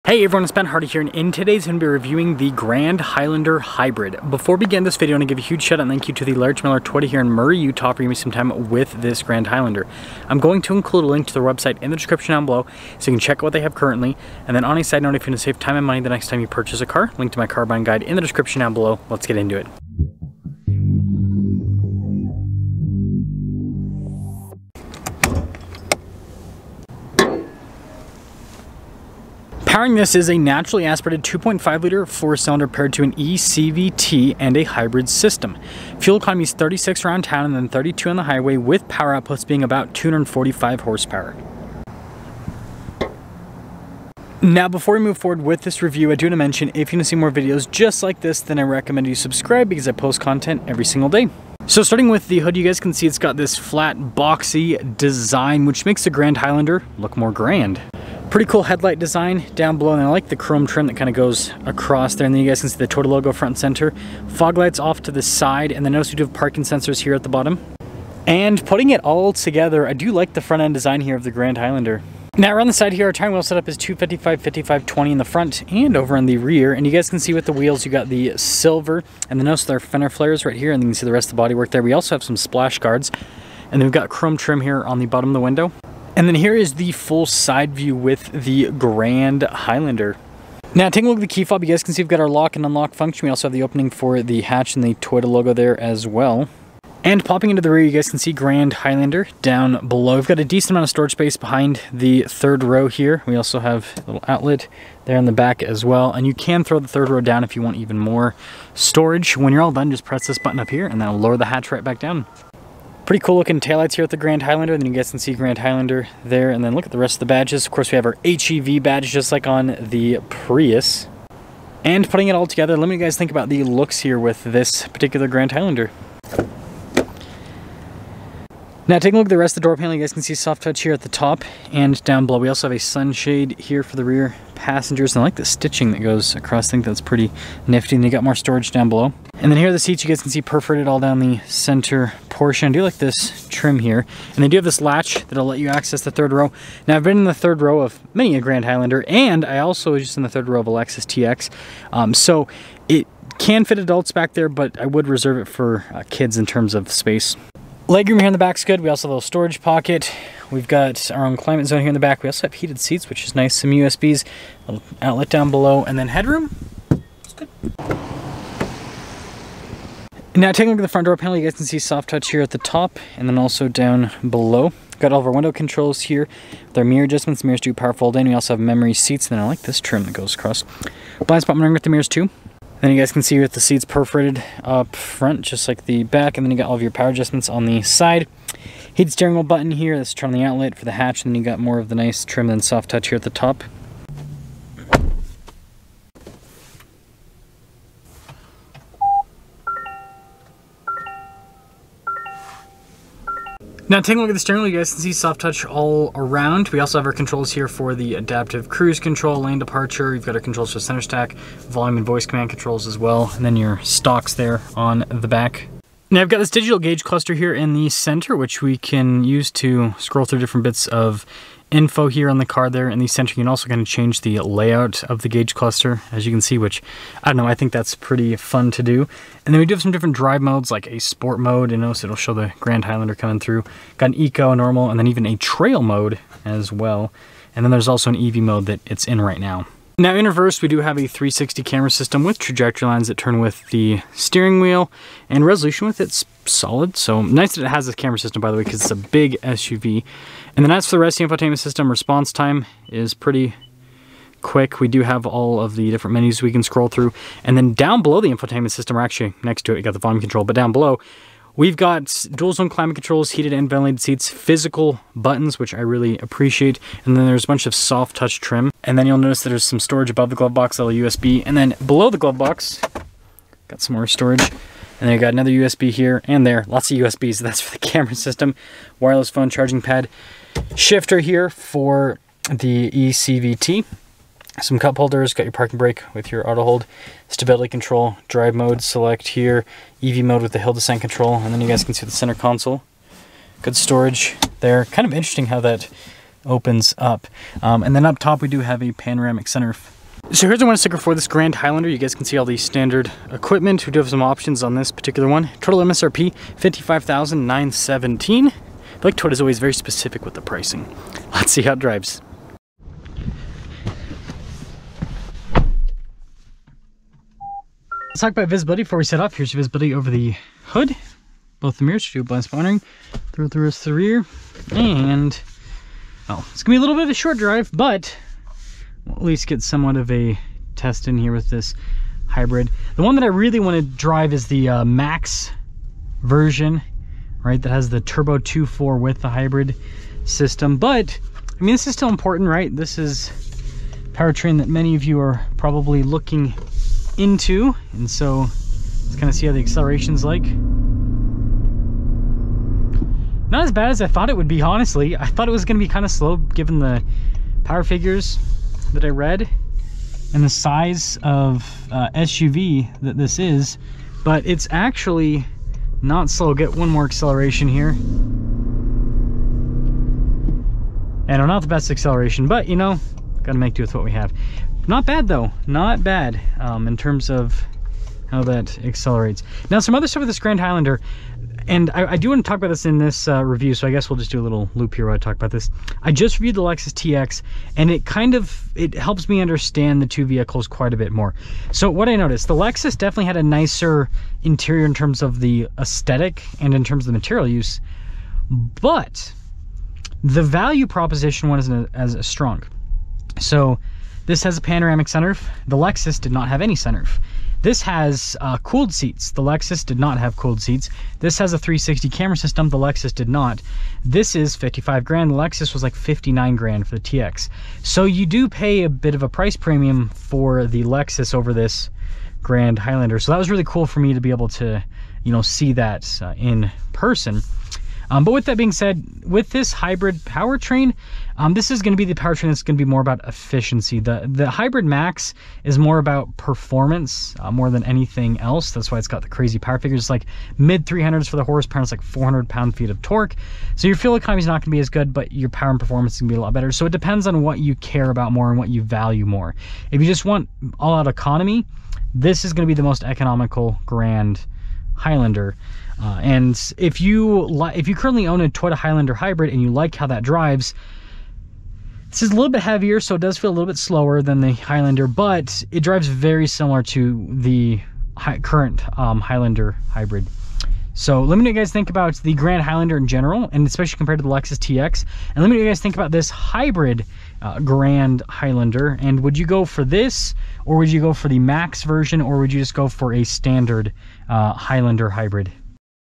2024 Toyota Grand Highlander (Hybrid): Is The Hybrid Worth It?
Today I drive and review a 2024 Toyota Grand Highlander Hybrid!